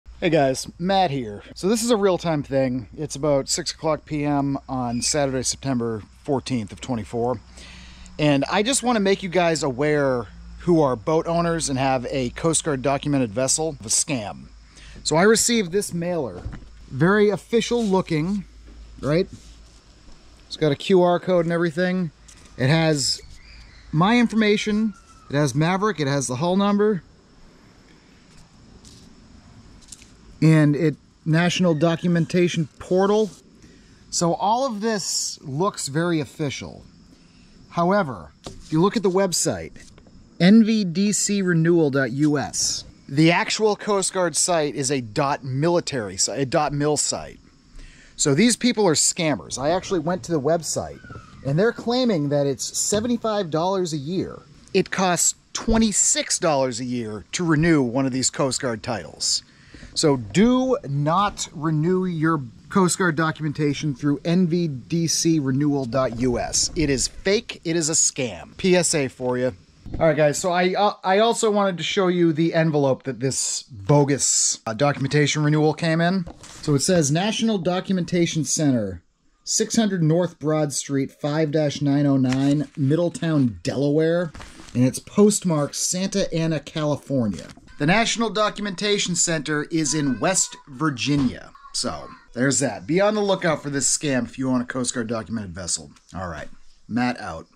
SCAM ALERT For Owners of USCG Documented Vessels!
A quick PSA about a scam I recently found out about. If you own a USCG documented vessel, watch this! Only renew your documentation through the official NVDC